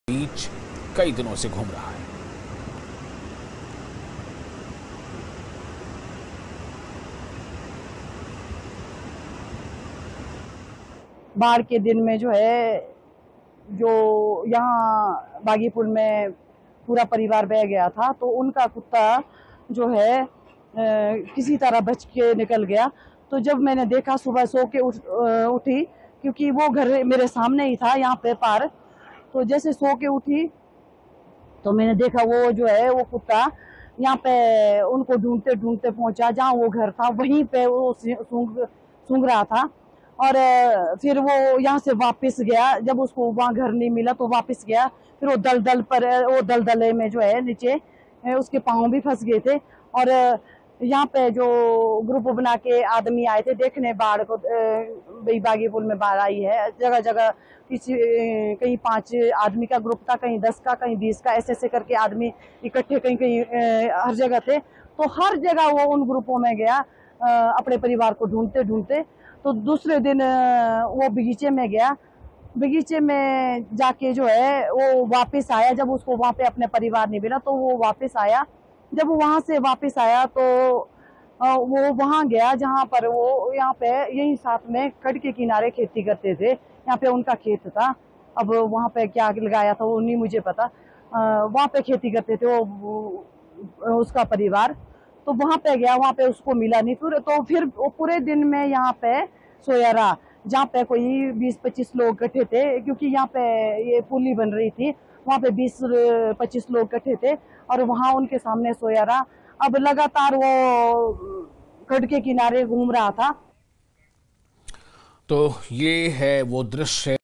Himachal weather Breaking :आपदा ने बेजुबानों पर कहर,तेज बहाव के बीच अपनों को ढूंढ रहे| hindi news
आपदा ने इंसान के साथ-साथ बेजुबानों पर भी ढाया कहर
तेज बहाव के बीच अपनों को ढूंढ रहा बेजुबान
हिमाचल आपदा ने इंसान के साथ साथ बेजुबानों पर भी कहर ढाया है। जहां एक तरफ लोग अपनों को तलाश कर रहे हैं तो वहीं एक रामपुर में एक अनोखा दृश्य देखने को मिला जहां एक बेजुबान कुत्ता अपनों की खोज में पानी की धार के बीच कई दिनों से घूम रहा है। बागीपुल में रहने वालों ने बताया कि हमारे लोग बह गए लेकिन किसी तरह ये बेजुबान कुत्ता बच गया । मैं जब सोकर उठी तो देखा कुत्ता अपनों को उन्हीं घरो में ढूंढ रहा था उसे घर नहीं मिला, जहां जहां भीड़ लगी थी वह वहां जा जाकर देख रहा था। वो अपने मालिक के खेत में भी गया लेकिन कोई नहीं मिला।
The disaster wreaked havoc on humans as well as the voiceless.
The voiceless man is searching for his loved ones amidst the strong current.
Himachal disaster has wreaked havoc not only on humans but also on the voiceless. While on one hand people are searching for their loved ones, on the other hand, a unique scene was seen in Rampur where a mute dog has been wandering in the water's edge for many days in search of its loved ones. The people living in Bagipul told that our people were washed away but somehow this voiceless dog survived.